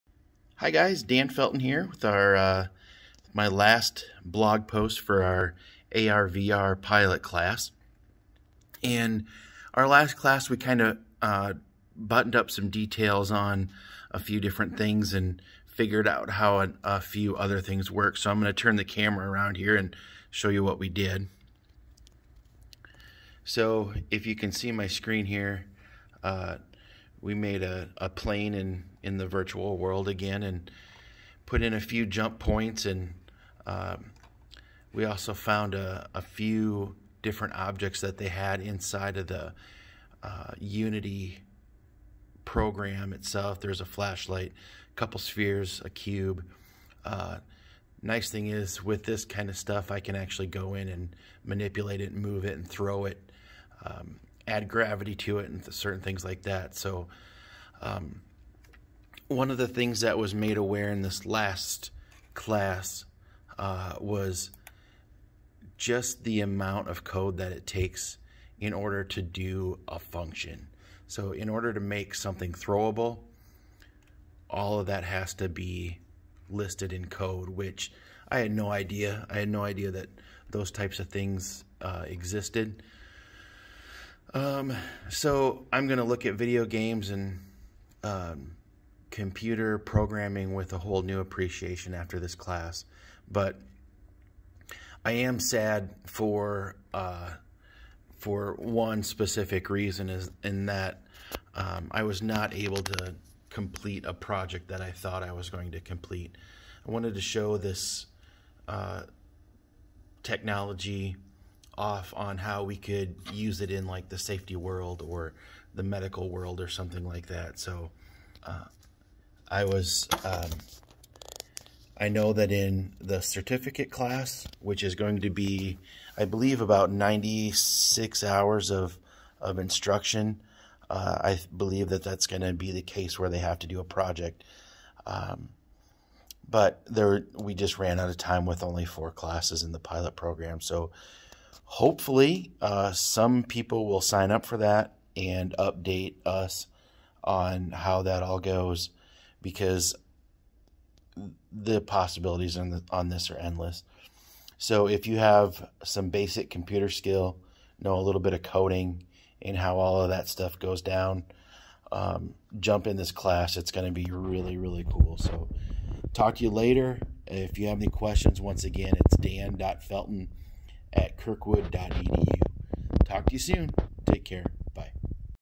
Dan Felton Virtual Reality Kirkwood Continuing Education Final class Blog
Dan shares his thoughts after the last class of the Continuing Education Virtual/Augmented reality class